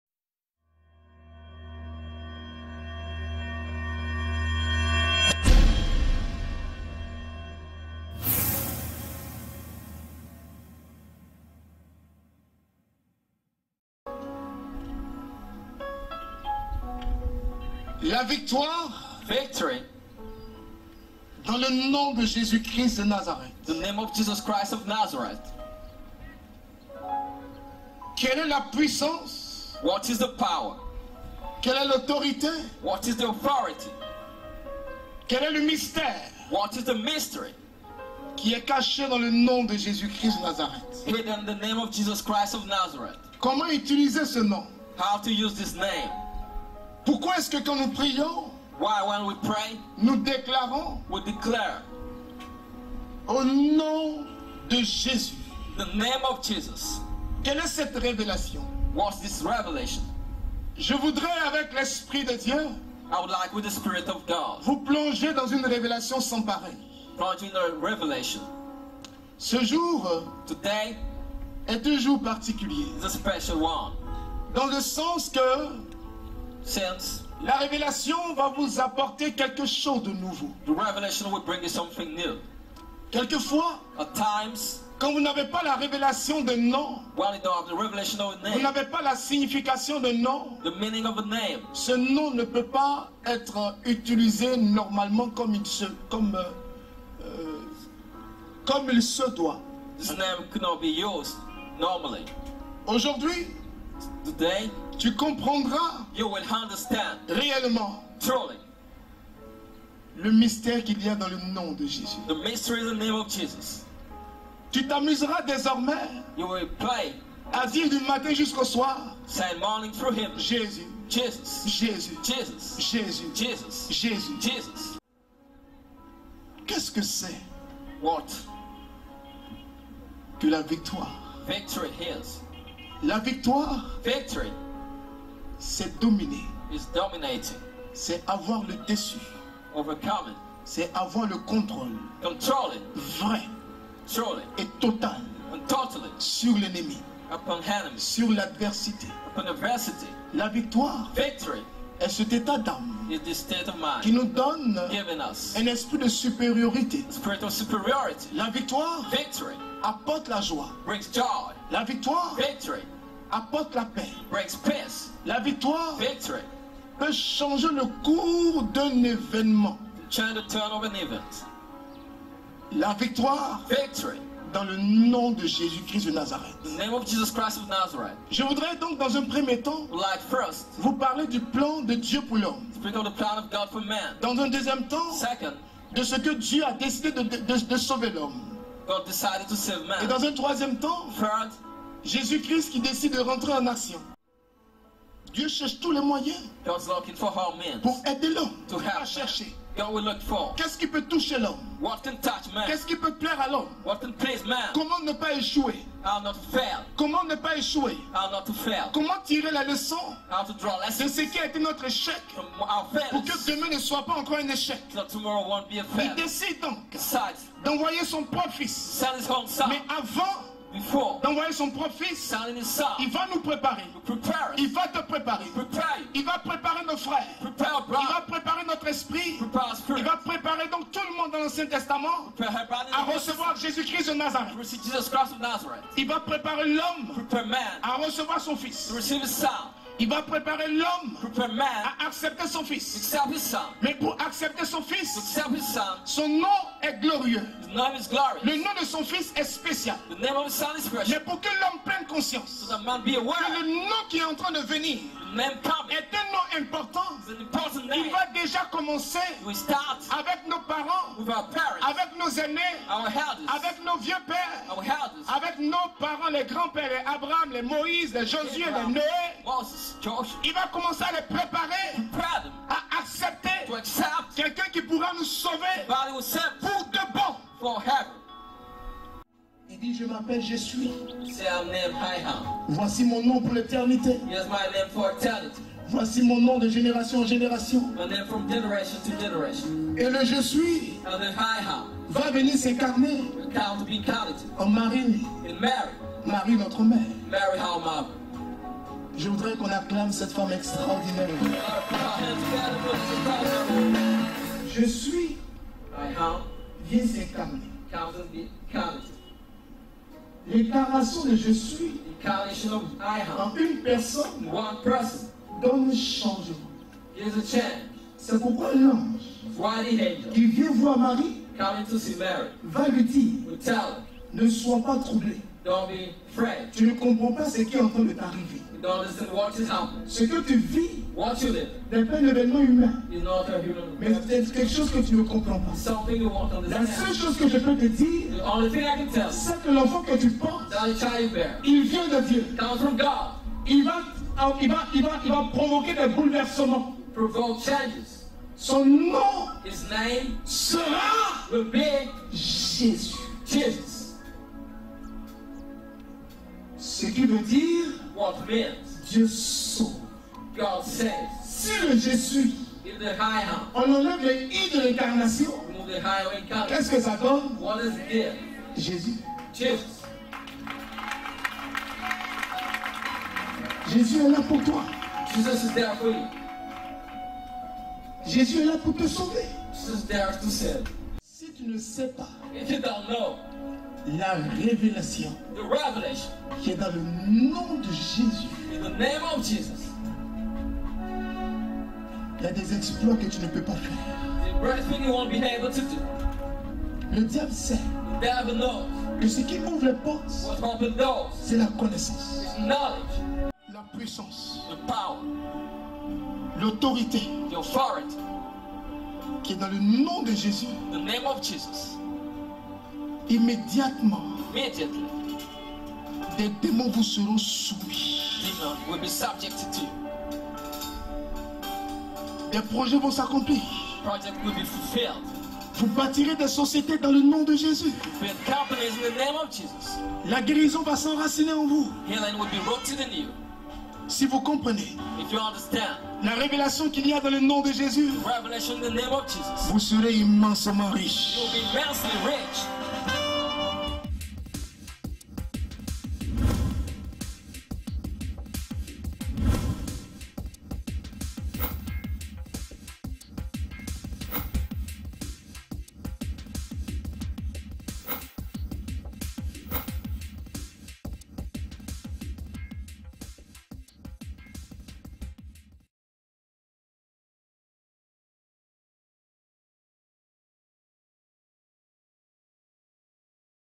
SACERDOCE ROYAL - RÉVÉLATION du Dim 26 AVRIL 2015 - Ens Mod. ABRAHAM MARIE
TEMPS DE PRIÈRE «RÉVÉLATION » DU DIMANCHE 26 AVRIL 2015

THÈME : «LA VICTOIRE DANS LE NOM DE JÉSUS »
(LUC 10,19)

Tous les Dimanches à partir de 14H à la Sphère de la bénédiction en face du chateau d'eau de Cocody Angré. Temps de prières, de louanges, d'enseignements.

***Enseignement du Modérateur Abraham Marie (Extrait Vidéo)

et (le site personnel du Modérateur Abraham Marie)